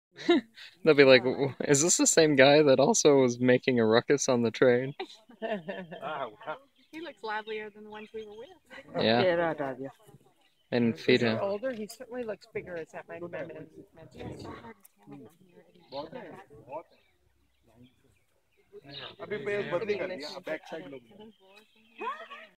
Big tiger doing his stretches
Out in the sun and working on his tan